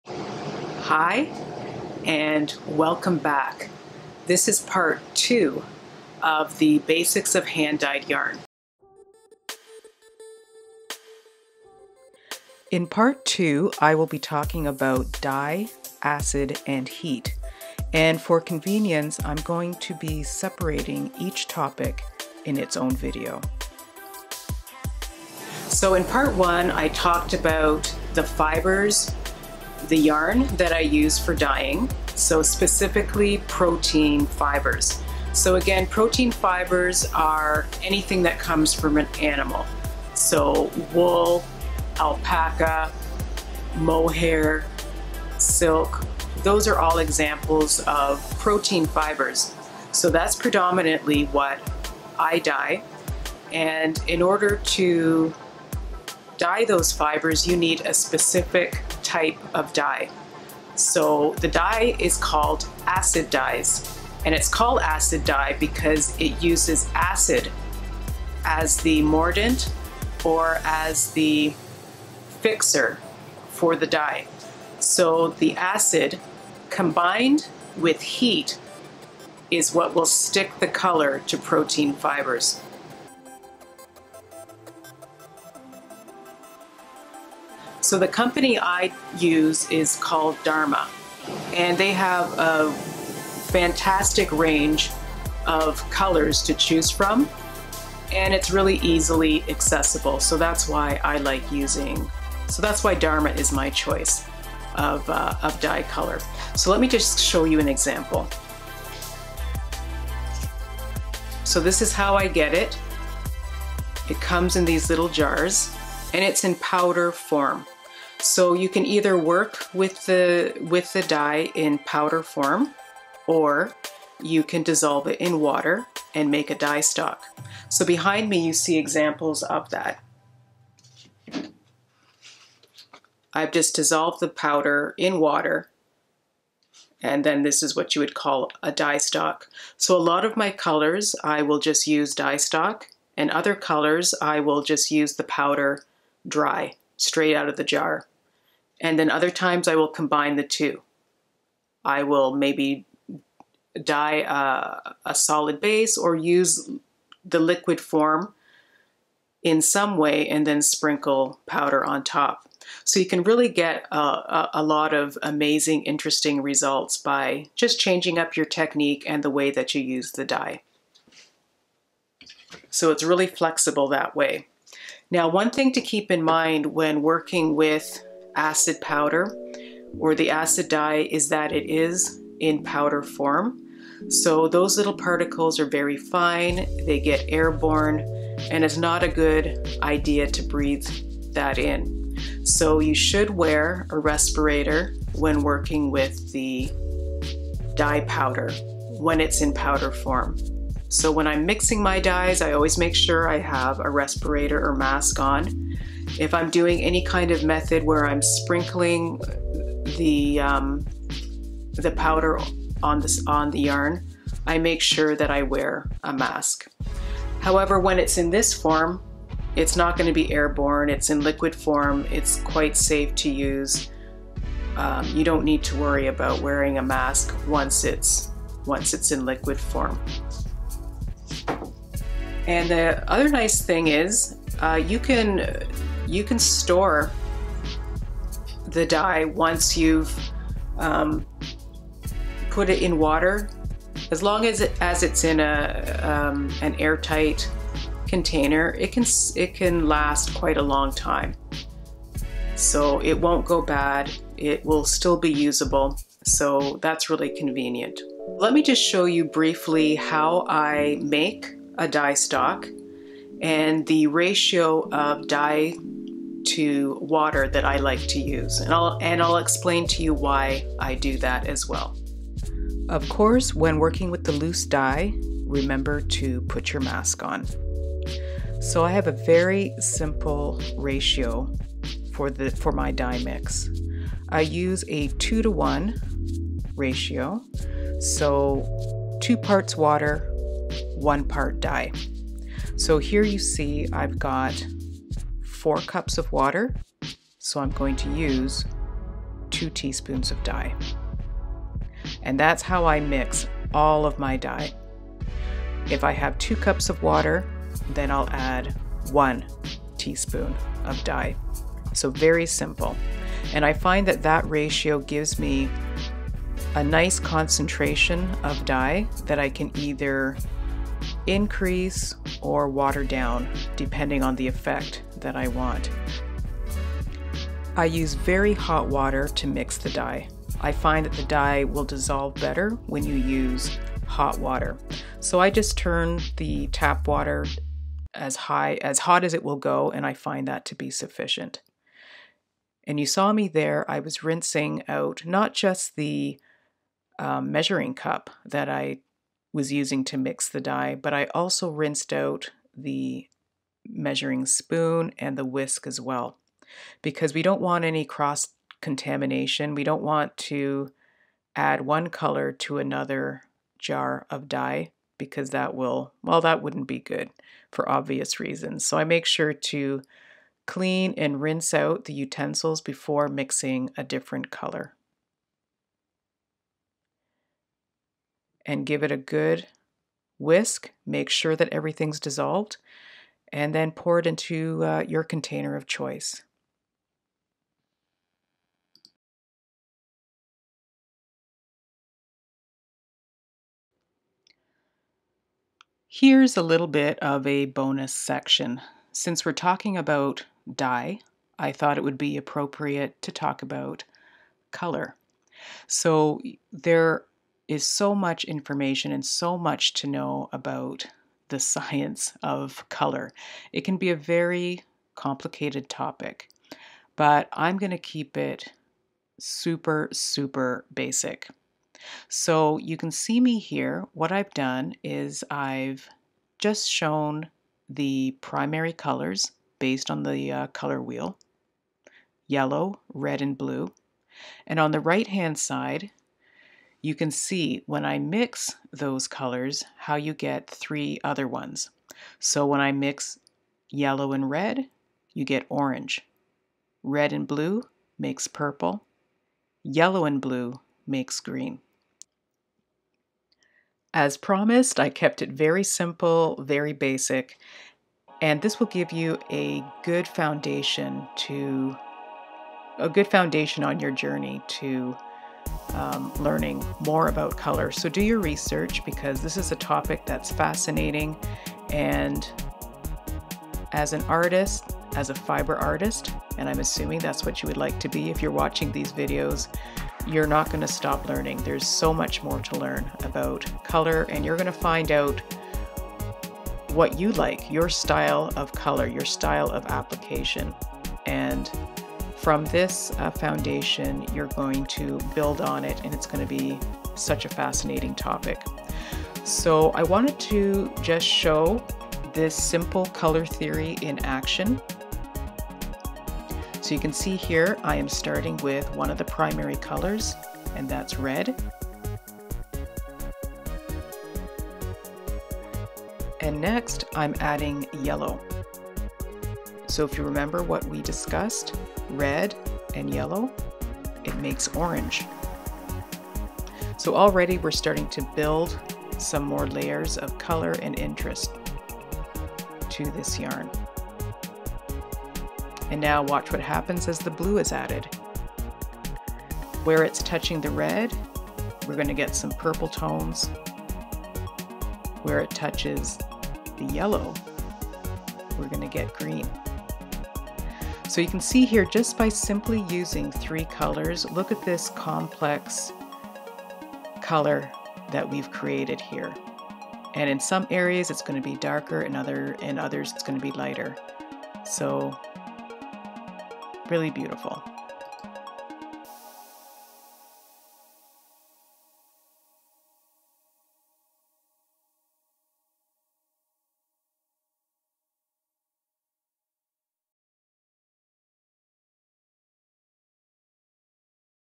The Basics Of Hand Dyed Yarn (Part 2 Section 1) - DYE
Hi and welcome back. This is Part 2 in a series of videos specifically about the techniques and methods I use when creating hand dyed yarn. However before the dyeing process begins there are a few things to know which I call the BASICS. These Basics include what you need to know before you get started dyeing yarn and will be the foundation to build on as you learn how to create your own unique works of art with fibre.

Part 1 discussed protein fibres and Part 2 will discuss the three major components you need to dye protein fibres, ACID, DYE and HEAT. Each component will have it's own separate video for convenience. This video will be about DYE.

For your convenience time stamps are listed in the info below so you can easily refer back to parts of the video you wish to review.

It encourages me to keep making videos and helps the channel to grow.

Sheryl

TIME STAMPS:
(0:31) What type of dye to use for protein fibres.
(1:57) Different ways to use acid dye.
(2:55) How to use acid dyes safely
(4:22) How to make a dye stock
(6:30) Basic colour theory